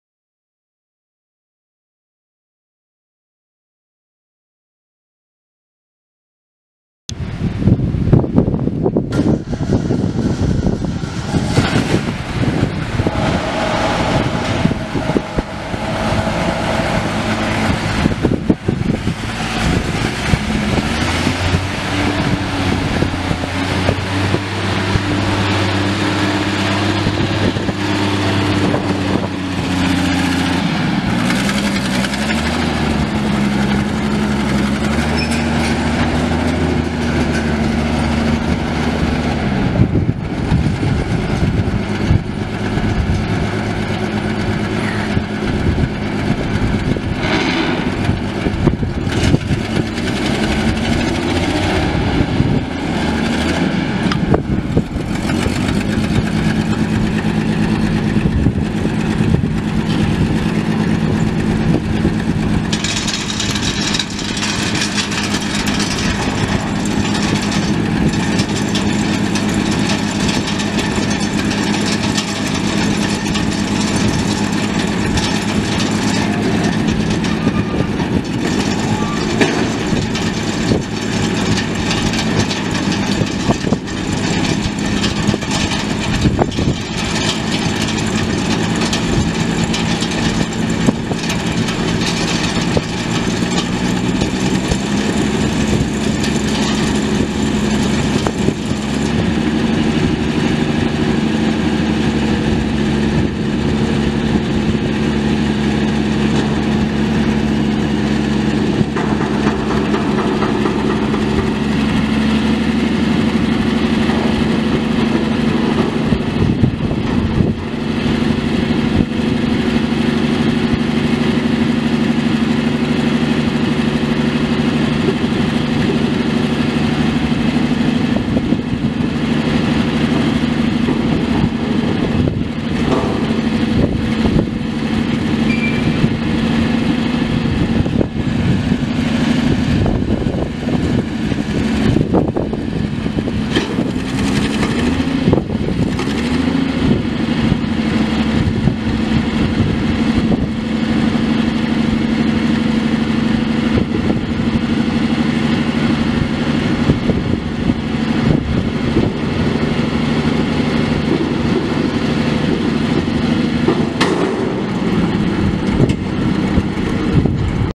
New Holland 10-47 self-propelled hay stacker for sale | no-reserve Internet auction July 26, 2017
New Holland 10-47 self-propelled hay stacker
Six cylinder propane engine
Four speed manual transmission
Canopy
Front and rear work lights
80"L x 9'W front deck
12'3"L x 9'W rear deck
(118) 14" bale capacity
(95) 16" bale capacity
11L-16.5 front tires
15-19.5 rear tires
Serial 13002